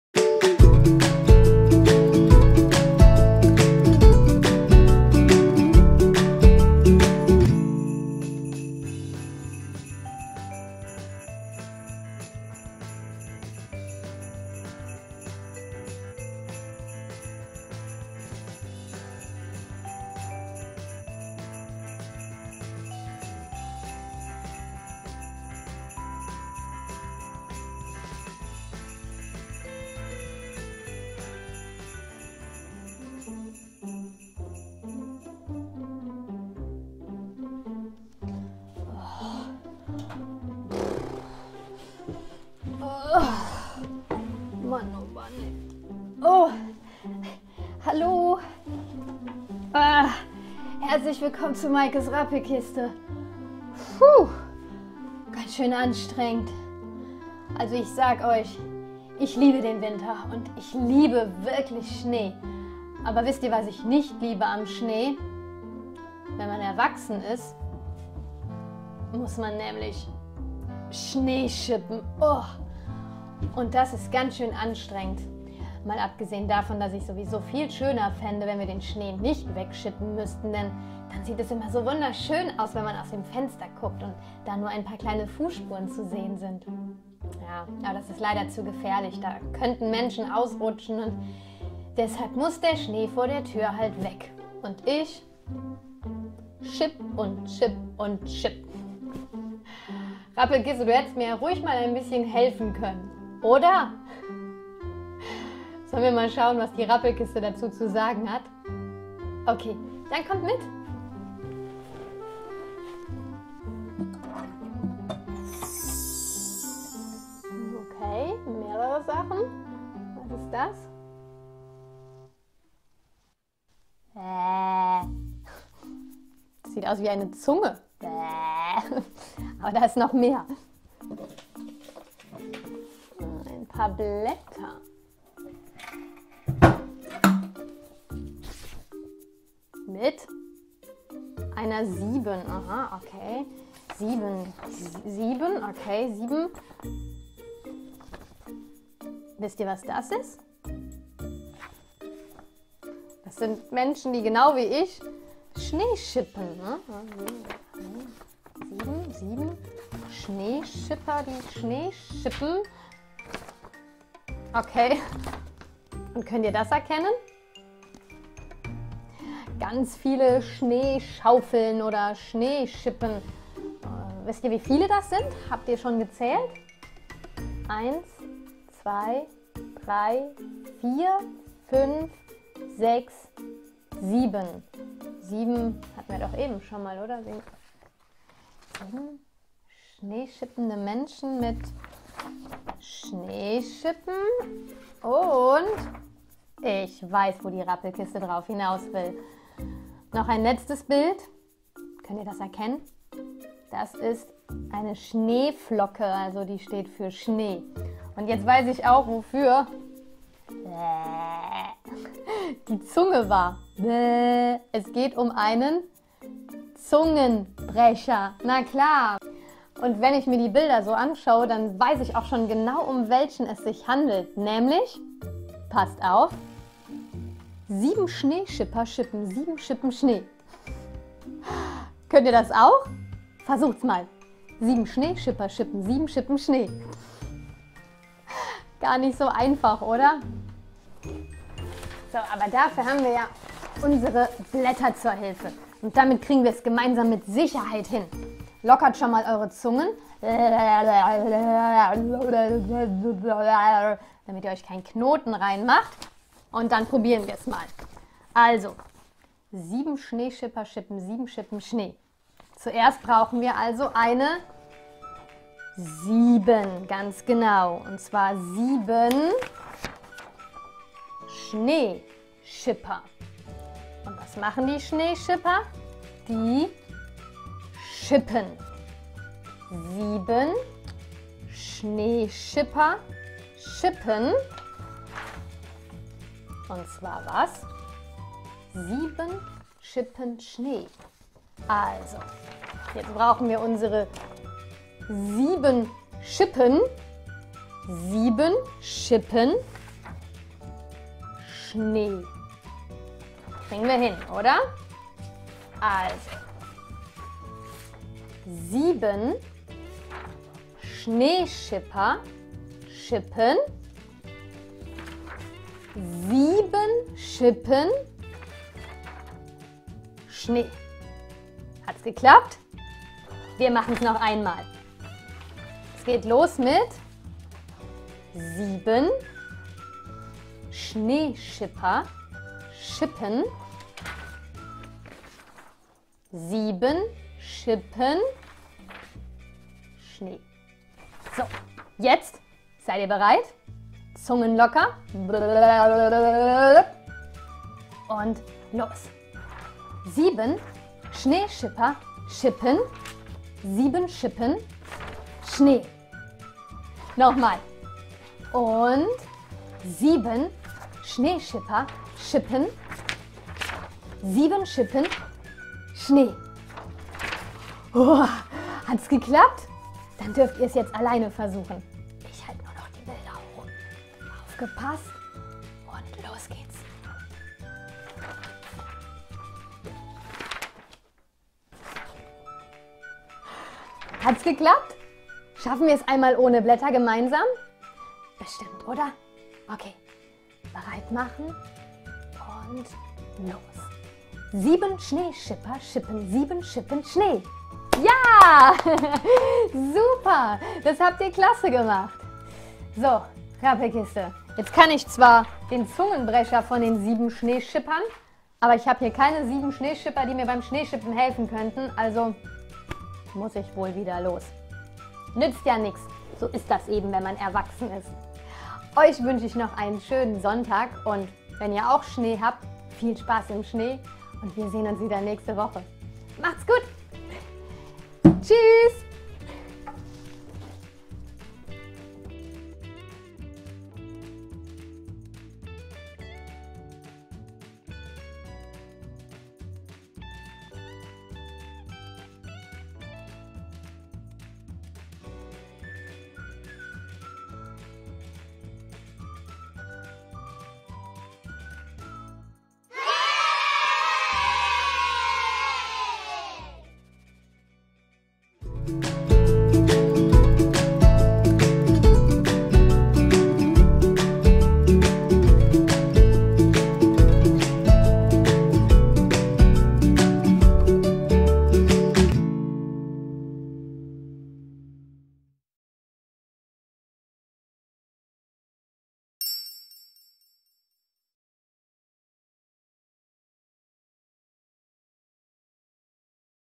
Ein Winter - Zungenbrecher für Kinder I Kindergarten I for Kidz I
Ein Zungenbrecher für Kinder im Winter

Ich liebe es, wenn es im Winter schneit. Ganz ehrlich! Alles in mir schreit dann: „Los, lass uns einen Schneemann bauen und eine Schneeballschlacht machen! Schmeiß´ dich in den Schnee und mach´ einen Schneeengel ;)“

Aber wisst ihr, was man als Erwachsener auch machen muss, wenn es geschneit hat?
Schneeschippen. Und das macht mir gar keinen Spaß.

Aber wenigstens hält die Rappelkiste einen lustigen, passenden Zungenbrecher bereit:

„Sieben Schneeschipper schippen sieben Schippen Schnee“

Wenn ihr Lust habt diesen mit mir zu üben, seid ihr in der heutigen Folge genau richtig.

Viel Erfolg und bis bald
Eure Maike